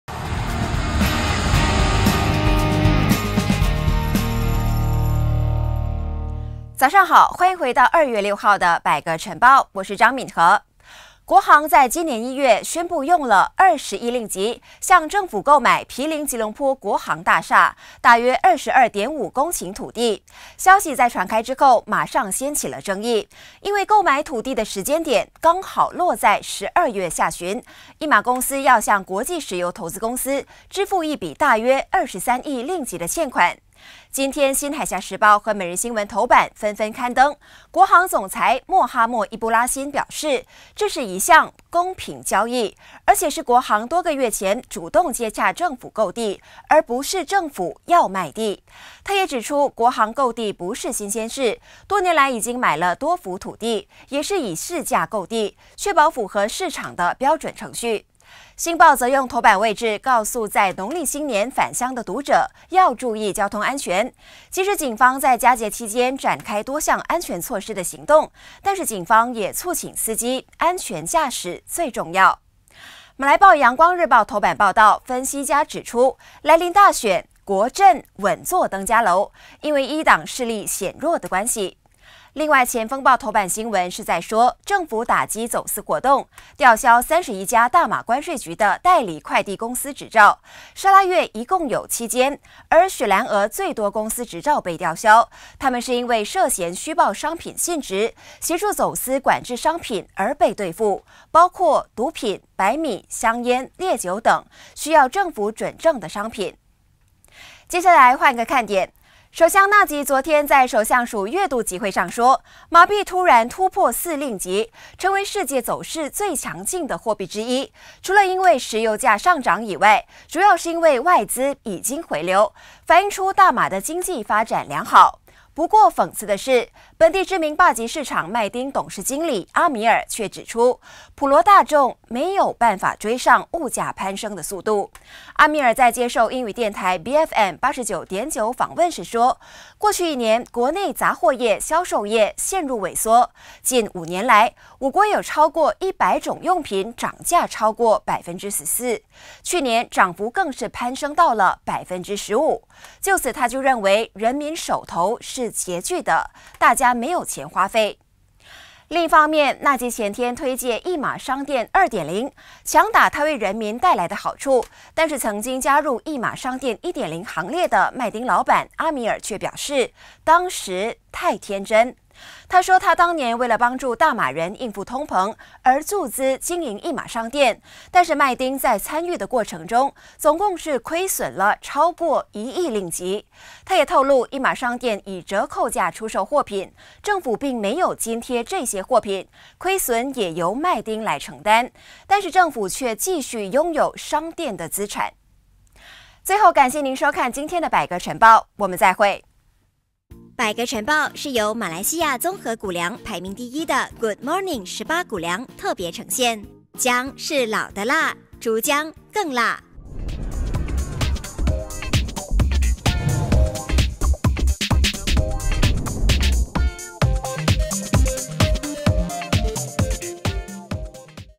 百货5年涨幅14%·Mydin董事经理：人民手头拮据
首相纳吉昨天月度集会上说，马币走势强劲，反映大马经济发展良好。不过本地知名霸级市场“迈丁”（MYDIN）董事经理阿米尔，却有不同看法.